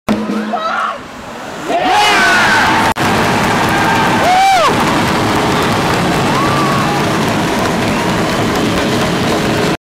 Devils vs Steelers, 9th September 2012. Mac Faulkner scores the first goal
Cardiff Devils vs Sheffield Steelers, 9th September 2012, Mac Faulkner scores the first goal.